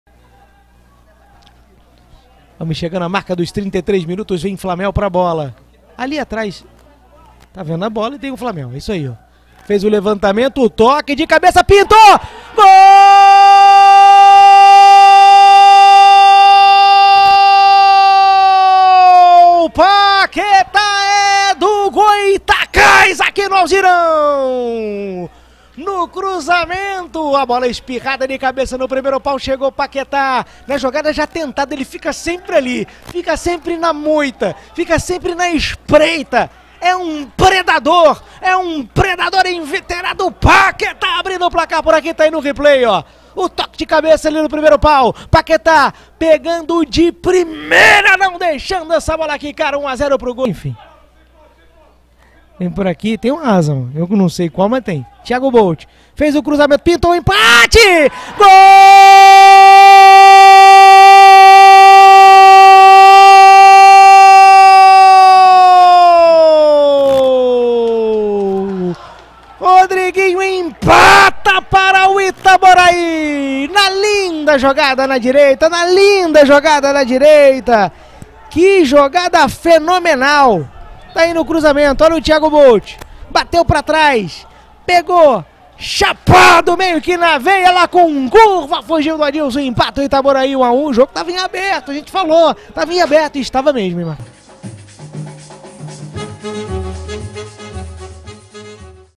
Estadual 2019 Série B1 - AD Itaboraí 1 x 1 Goytacaz - Taça Corcovado - 7ª Rodada
Estadual 2019 Série B1 - Taça Corcovado - 7ª Rodada